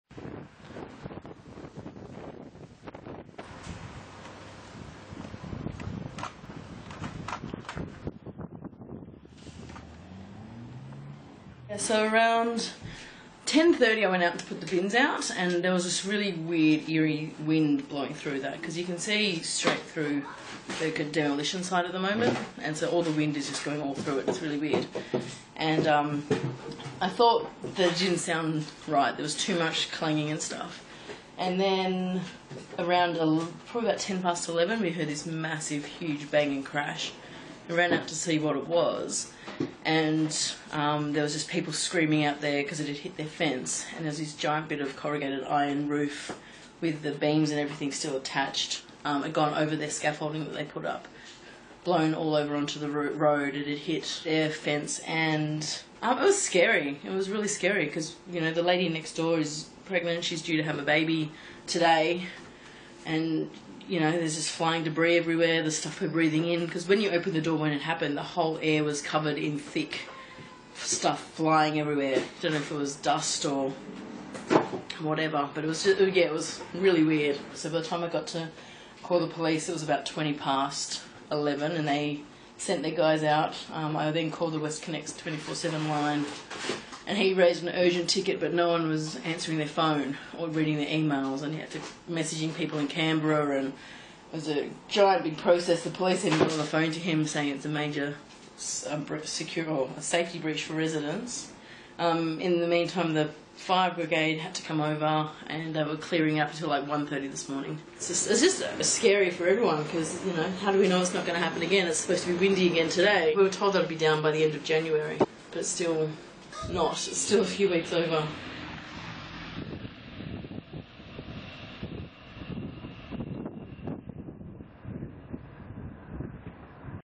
WestCONnex St Peters safety breach
A resident explains what happened on the night of 11 February 2017, when iron roofing was blown out of a WestCONnex demolition site in St Peters, damaging property and placing people's lives at risk.